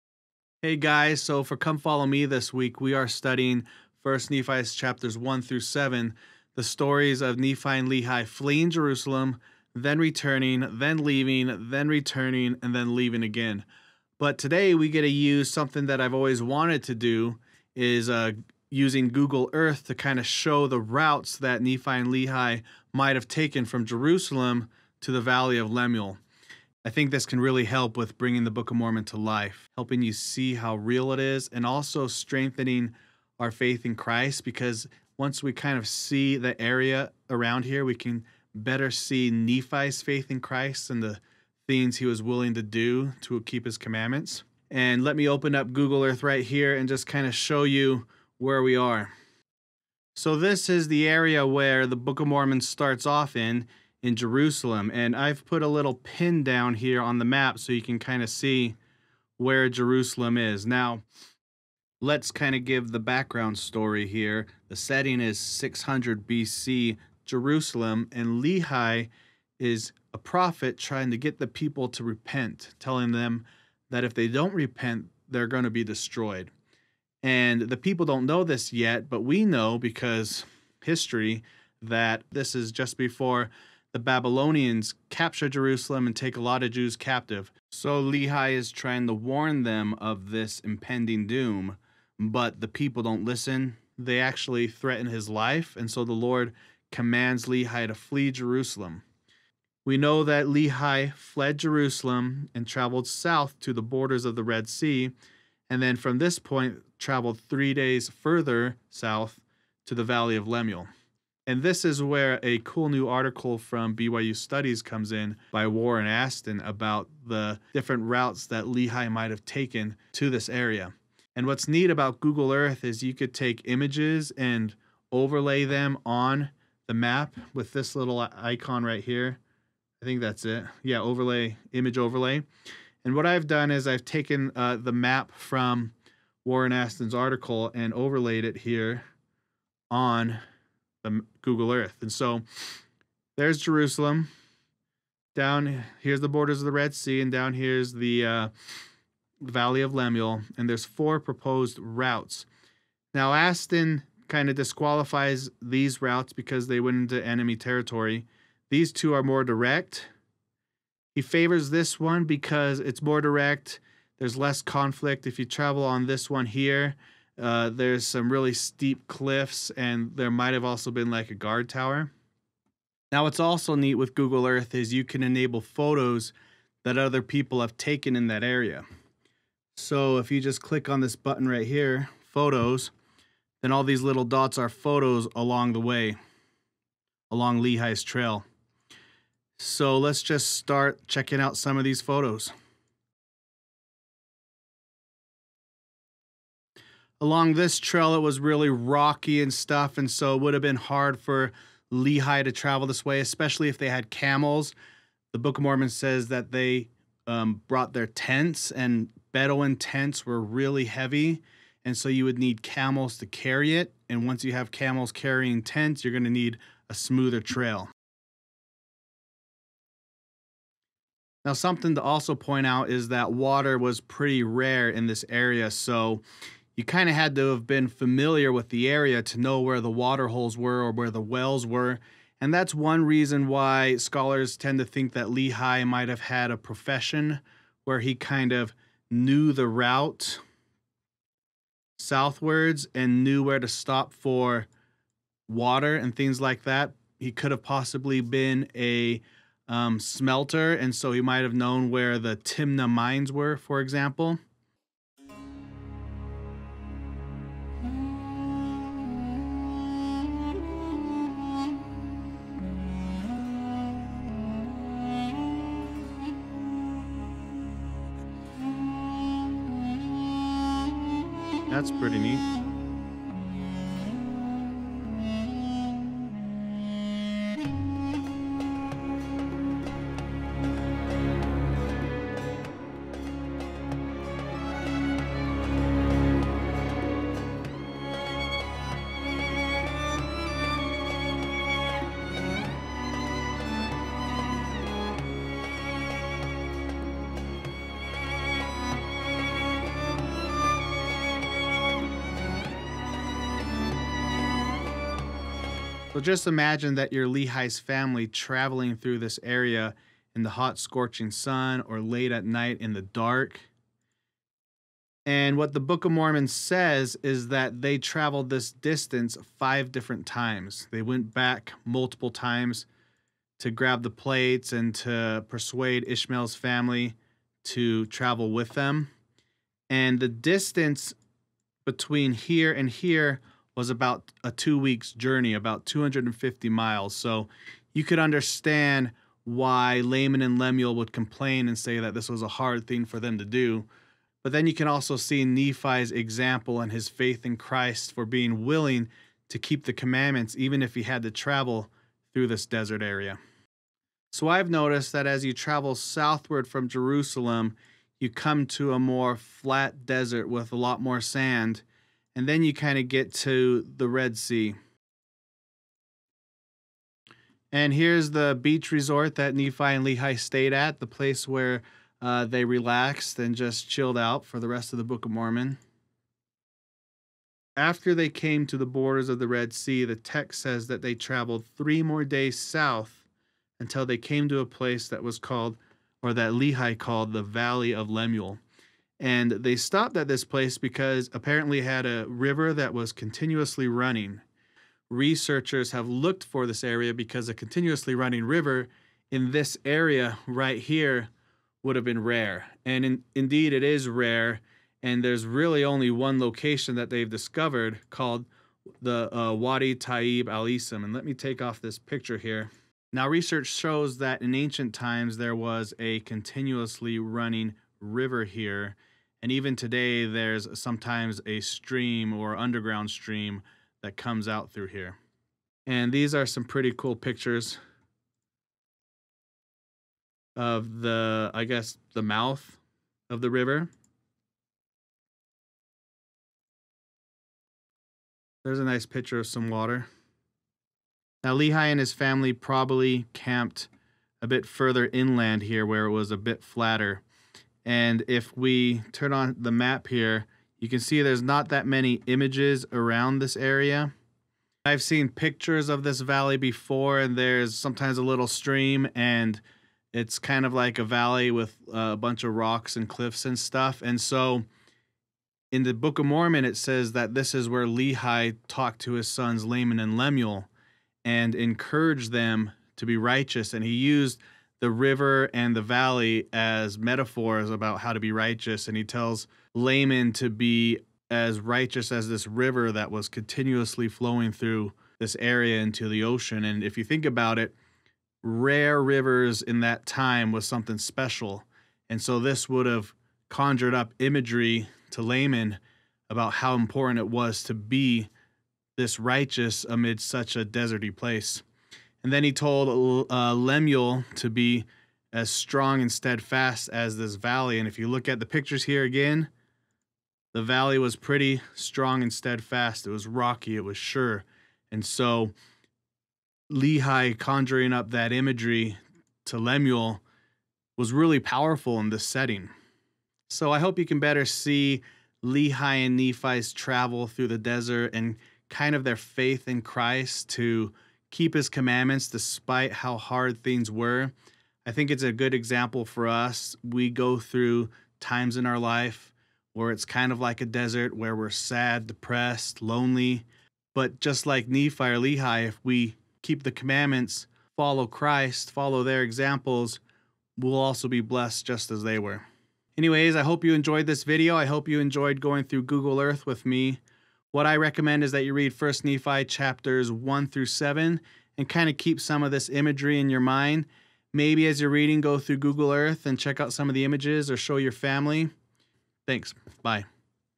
The Book of Mormon with Google Earth! (Valley of Lemuel)
Book of Mormon Central employee Zander Sturgill goes through the beginning chapters of the Book of Mormon using Google Earth, explaining Lehi and Nepih's travels from Jerusalem to the Valley of Lemuel.

Link to BYU Studies article. Click the PDF to view the images.